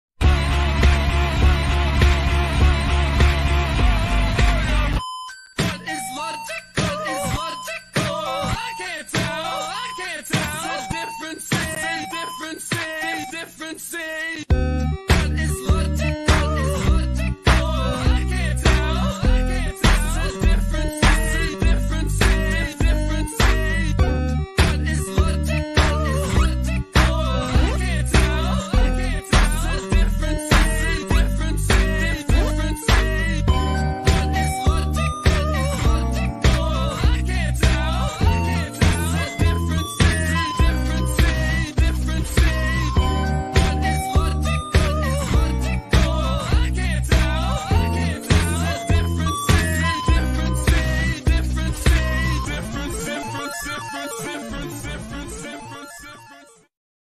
《WHAT IS LOGICAL?》《Meme》
⚠️ THESE ARENT AMONG US OCS THESE ARE CREATURES MADE BY ME AROUND 2015/2016 AND AMONG US WAS FIRST POSTED IN 2018! PLS REMEMBER THESE ARE FAIR-USE OCS- ⚠️

Pls don't copy-
steal-
Trace-
repost-
at all... (without permission)

These are new OCs named Nova & Nebula!
Their nemesis with Rockster & Rockstar